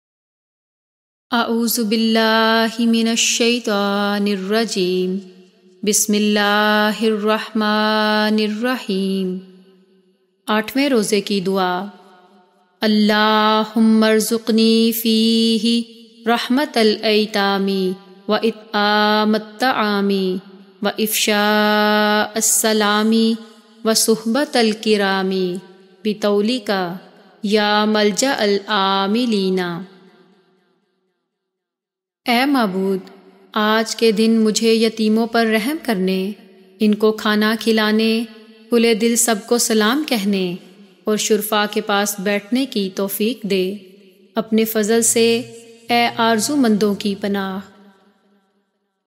Aathvain Rozay Ki Dua | 8th Of Ramadan Prayer | For Special Ramadan | Beautiful Life With NJ
Aathwain rozy ki dua.
In this holy month of Ramadan beautiful Life With NJ is presenting a Very beautiful and very special  segment which is Ramadan prayer￼.
Ramazan kay dinon ki duain.
In this video you are listen Aathvain rozay ki dua.
Eighth of Ramadan prayer.

For specially Ramadan. You Can Watch Very Beautiful And Informative Videos Only On Your favourite Youtube Chanal Beautiful Life With NJ